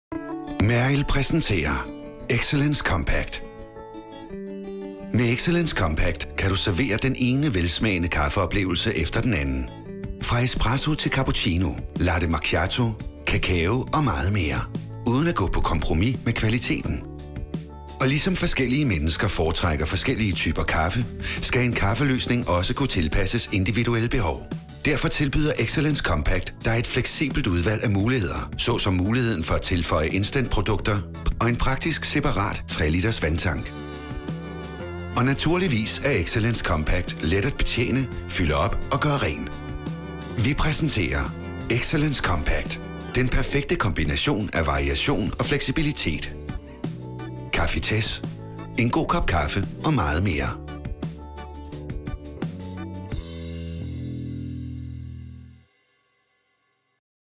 Excellence Compact fra Cafitesse
Cafitesse lanserer ny maskin - Excellence Compact!